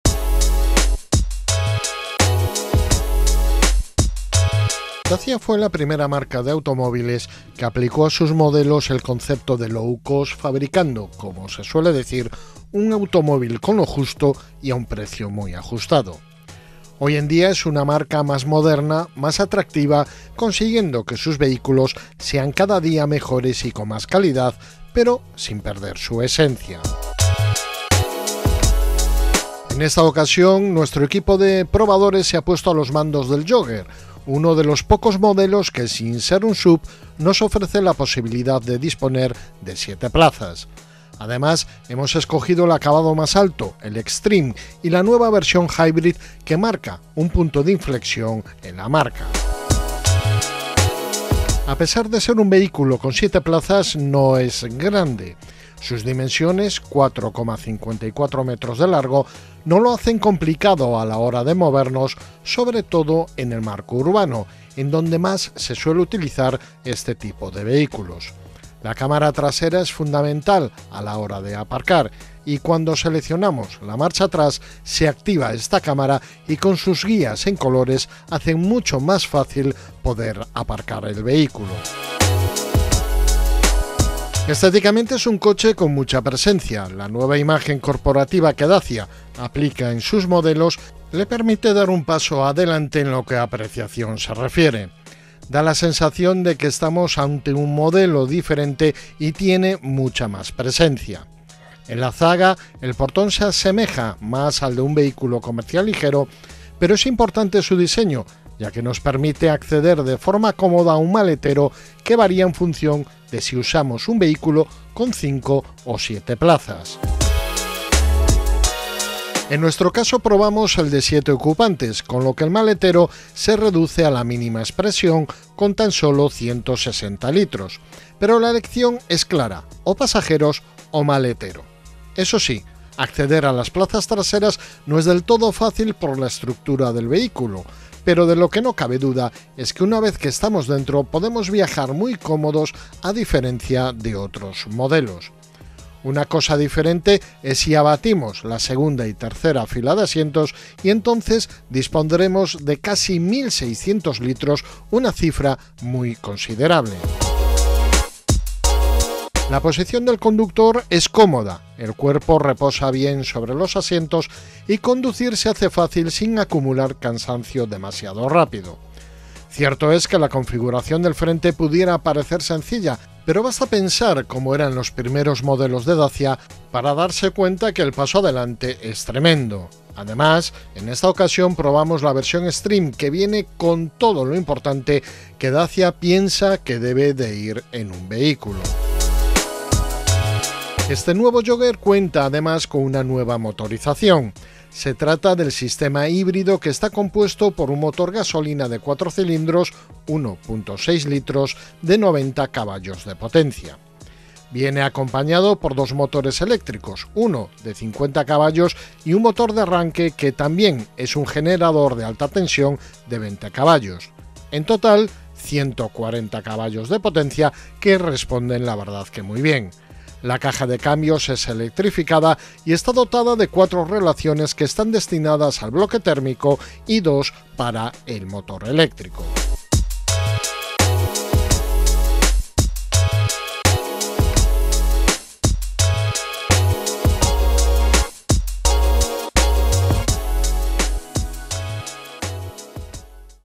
DACIA JOOGER HIGHLIGHTS&ACTIONS | By CdRas
Dacia fue la primera marca de automóviles que aplicó a sus modelos el concepto de “low cost” fabricando, como se suele decir, un automóvil con lo justo y un precio muy ajustado. Hoy en día es una marca más moderna, más atractiva, consiguiendo que sus vehículos sean cada día mejores y con más calidad, pero sin perder su esencia.
En esta ocasión nos hemos puesto a los mandos del Jogger, uno de los pocos modelos que sin ser un SUV nos ofrece la posibilidad de disponer de siete plazas. Además, hemos escogido el acabado más alto, el Extreme, y la nueva versión hybrid, que marca un punto de inflexión en la marca.
A pesar de ser un vehículo con siete plazas, no es grande. Sus dimensiones, 4,54 metros de largo, no lo hacen complicado a la hora de movernos, sobre todo en el marco urbano, en donde más se suele utilizar este tipo de vehículos. La cámara trasera es fundamental a la hora del aparcamiento. Cuando seleccionamos la marcha atrás, se activa la cámara, y con sus guías en colores hacen fácil poder aparcar.
Estéticamente es un coche con mucha presencia. La nueva imagen corporativa, que Dacia aplica en este modelo, le permite dar un paso adelante en lo que a apreciación se refiere. Da la sensación de que estamos ante un modelo diferente y tiene mucha más presencia. En la zaga, el portón se asemeja más al de un vehículo comercial ligero, pero es importante su diseño ya que nos permite acceder de forma cómoda a un maletero que varía en función de si usamos un vehículo con cinco o siete plazas.
En nuestro caso probamos el de siete ocupantes, con lo que el maletero se reduce a la máxima expresión, con 160 litros. Pero la elección es clara, o pasajeros o maletero. Eso sí, acceder a las plazas traseras no es del todo fácil por la estructura del vehículo, pero de lo que no cabe duda es que una vez que estamos dentro, podemos viajar muy cómodos, a diferencia de otros modelos. Una cosa diferente es si abatimos la segunda y tercera fila de asientos. Entonces dispondremos de casi 1.600 litros, una cifra muy considerable.
La posición de conducción es cómoda. El cuerpo reposa bien sobre los asientos, y conducir se hace fácil sin acumular cansancio demasiado rápido. Cierto es que la configuración del frente pudiera parecer sencilla, pero basta pensar cómo eran los primeros modelos de Dacia para darse cuenta que el paso adelante es tremendo. Además, en esta ocasión probamos la versión Extreme, que viene con “todo” lo importante que Dacia piensa que debe ir en un vehículo.
Este nuevo Jogger cuenta, además, con una nueva motorización. Se trata de un sistema híbrido que está compuesto por un motor gasolina de cuatro cilindros, 1.6 litros, de 90 CV de potencia. Viene acompañado por dos motores eléctricos, uno de 50 CV, y un motor de arranque que también es un generador de alta tensión, de 20 CV. En total, 140 CV, que responden muy bien. La caja de cambios es electrificada, y está dotada de cuatro relaciones que están destinadas al bloque térmico, y dos para el eléctrico.